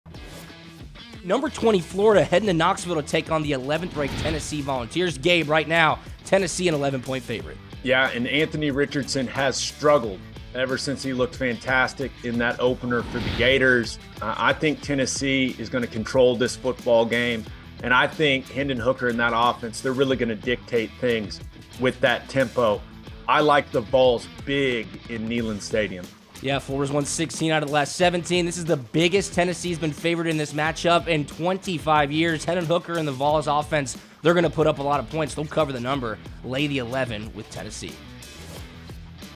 Biggest spread in 25 Years for Gators & Vols!👀💥
Gabe Ikard joins Drew Butler to share his expert predictions on this weekend's big College football matchup: Florida vs Tennessee.

Earn a 50% Welcome Bonus up to $1,000 on your first-ever deposit at BetOnline.